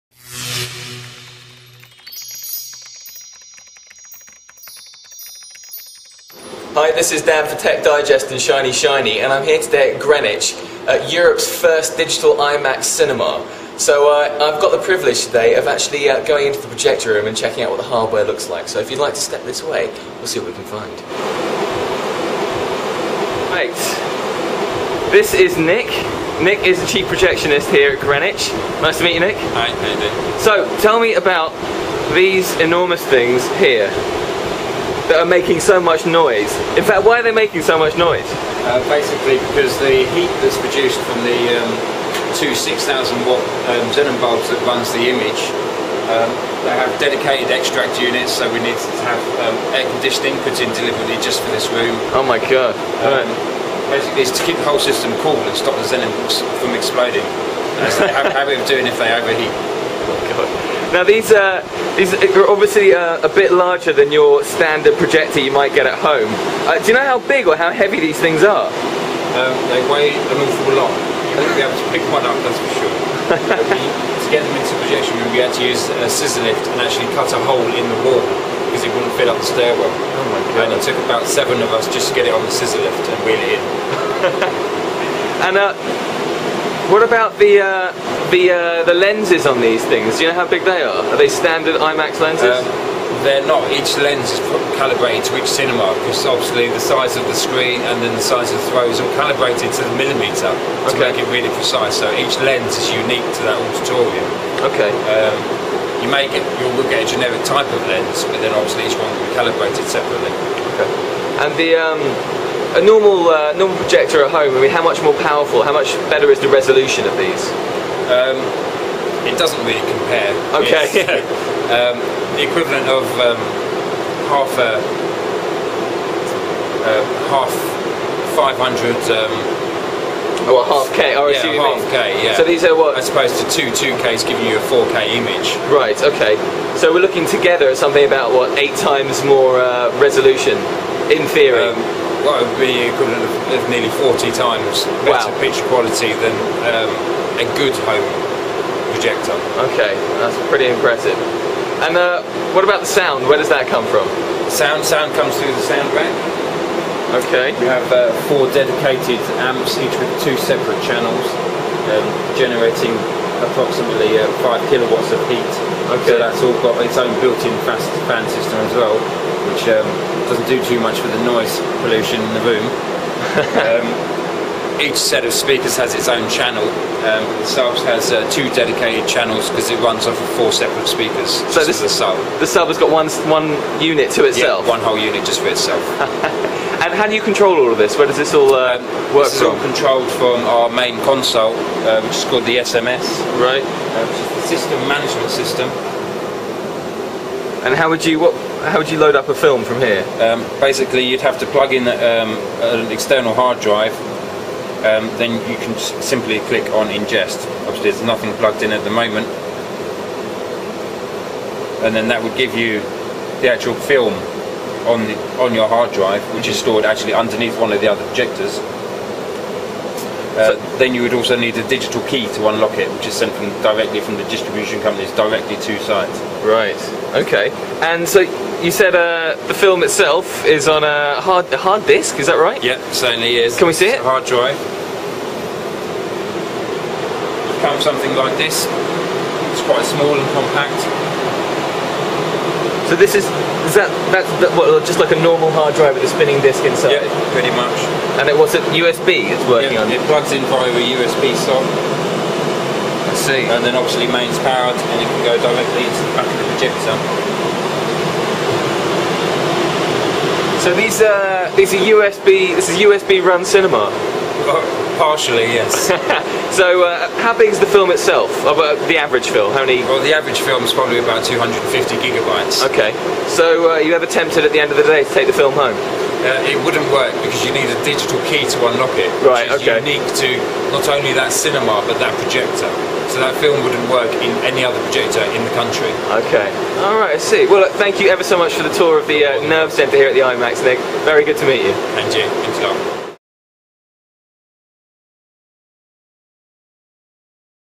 Shiny Media: The first Digital Imax Projection in Greenwich
Dan looks into Imax digital projection.